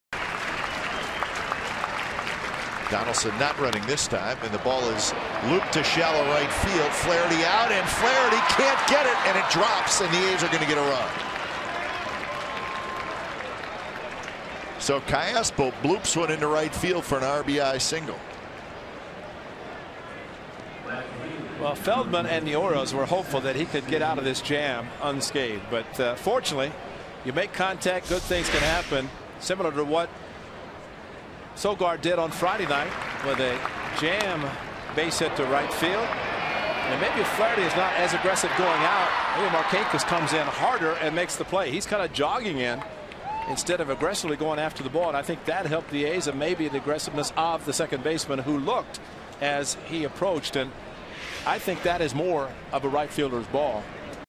2013/08/25 Callaspo's RBI single
OAK@BAL: Callaspo drives in Crisp for the first run